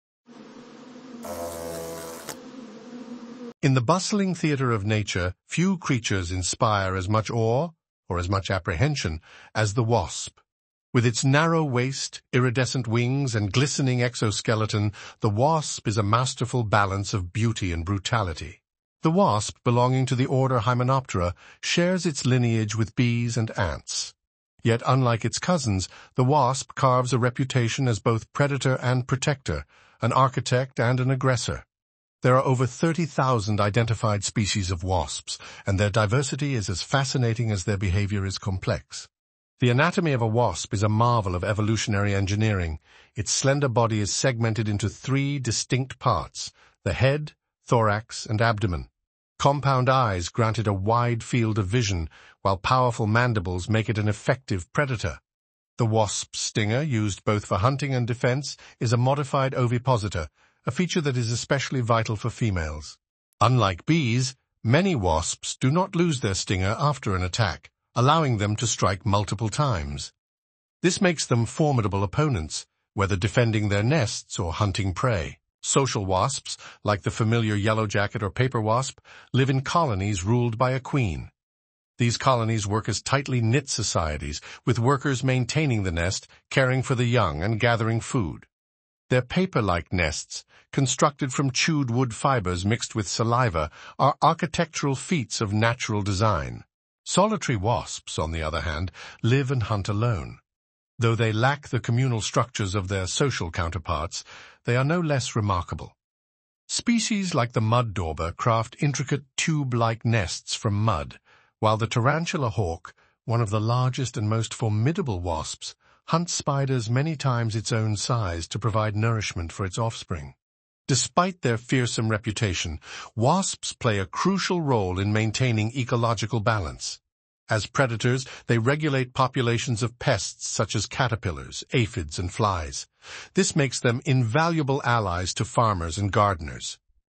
The Characteristics of Wasp
Wasps aren’t just scary — they’re way more dangerous and fascinating than you think! In this video, we reveal shocking facts about wasps that most people have NEVER heard of!

Perfect for students, nature lovers, or anyone curious about these fascinating insects!